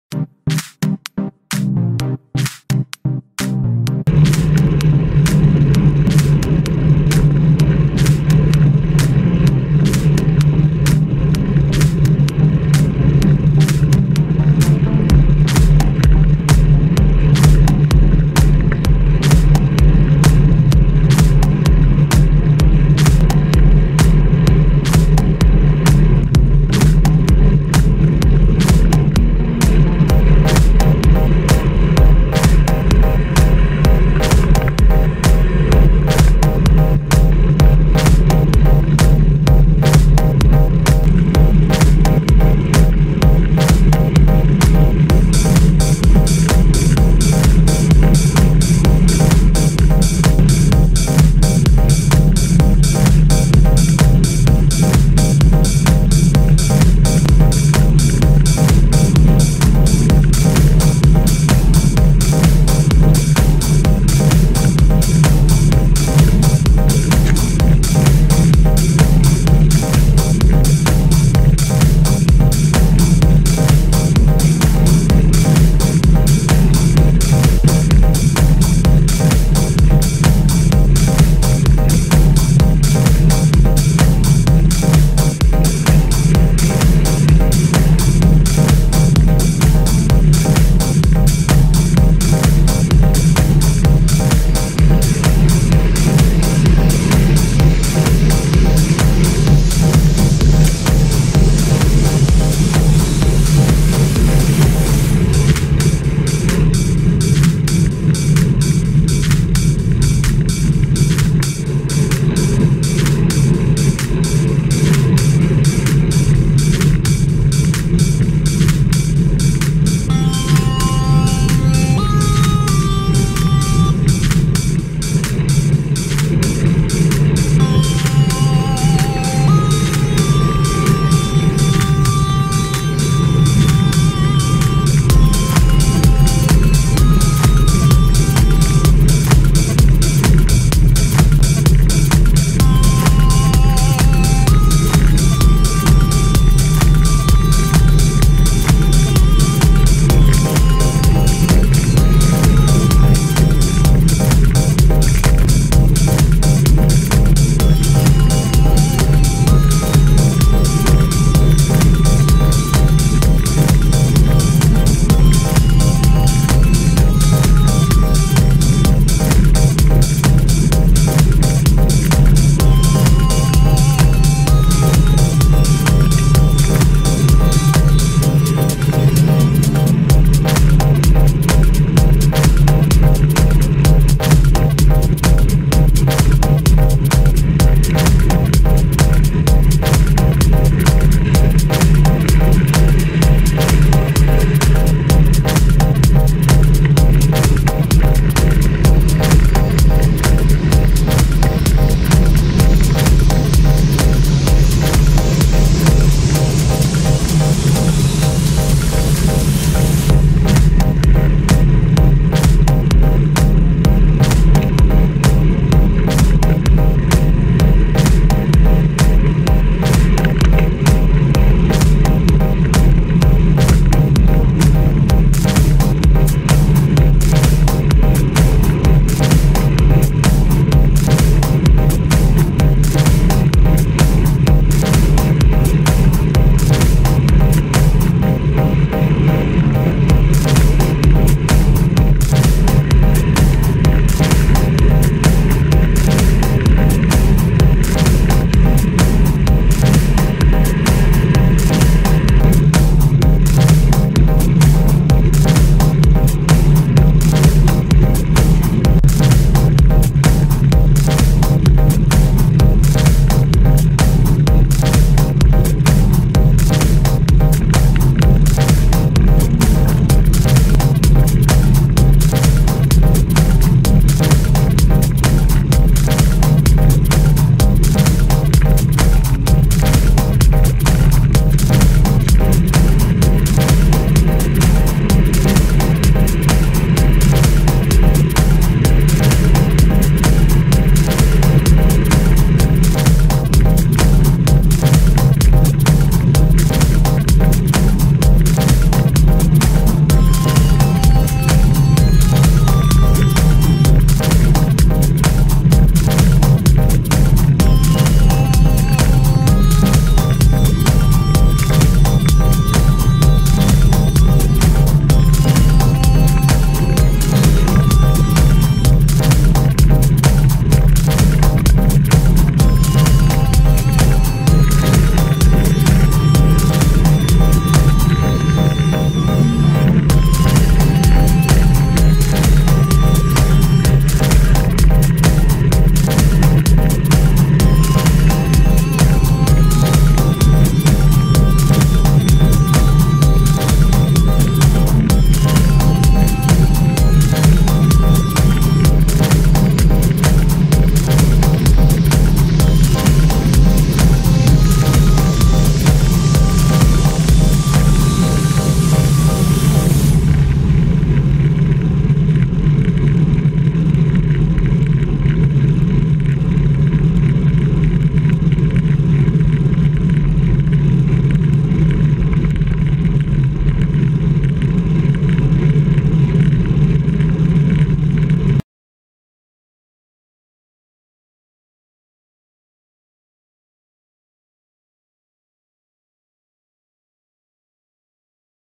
Cycling - rider view - Langside / Comrie district - Misty March
DescriptioSome video. view of cycling around Auchterarder, Langside, Comrie area of Perthshire